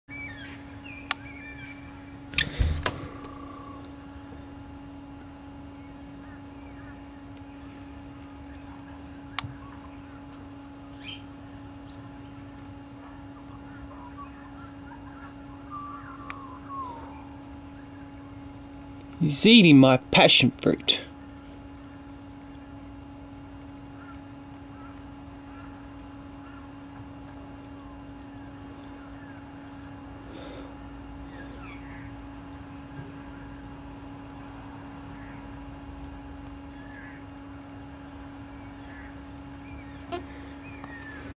PassionFruit Thief !!
This is a small  video I did sometime ago for proof of who keeps stealing my passionfruits from the vines.